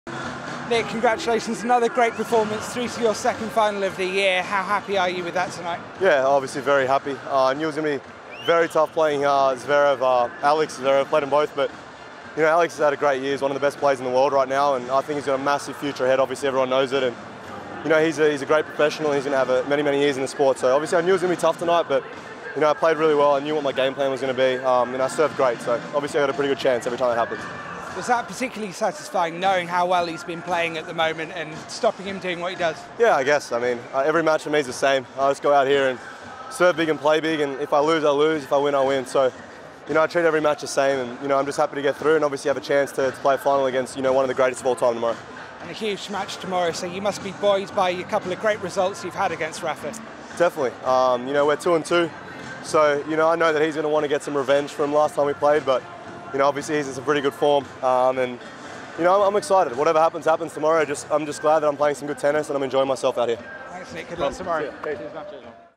Kyrgios Reflects On Zverev Win In Beijing 2017
Nick Kyrgios discusses his semi-final victory over Alexander Zverev and looks ahead to the Beijing final against Rafael Nadal. Watch live tennis at tennistv.com.